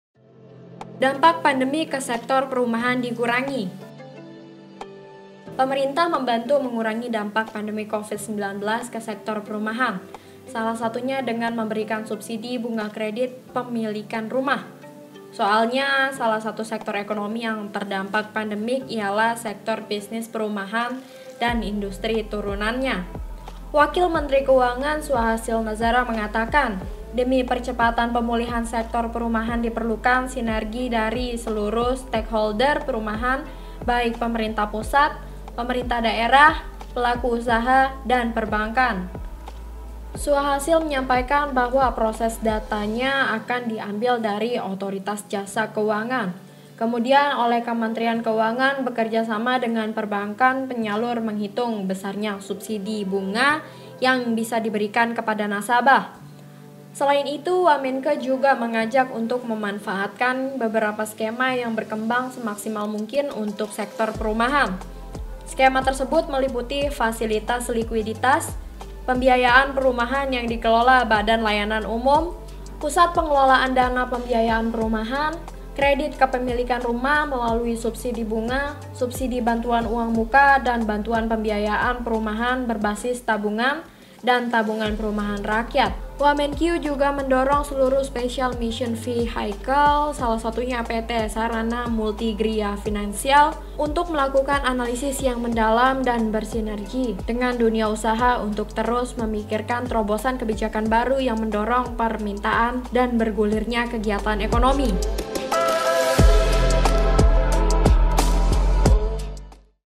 Dampak Pandemi Covid-19 ke Sektor Perumahan Dikurangi
JAKARTA – Pemerintah membantu mengurangi dampak Pandemi Covid-19 ke sektor perumahan. Salah satunya dengan memberikan subsidi bunga kredit pemilikan rumah (KPR). Soalnya, salah satu sektor ekonomi yang terdampak pandemi ialah sektor bisnis perumahan dan industri turunannya.

Wakil Menteri Keuangan (Wamenkeu), Suahasil Nazara mengatakan, demi percepatan pemulihan sektor perumahan diperlukan sinergi dari seluruh stakeholder perumahan baik pemerintah pusat, pemerintah daerah, pelaku usaha, dan perbankan.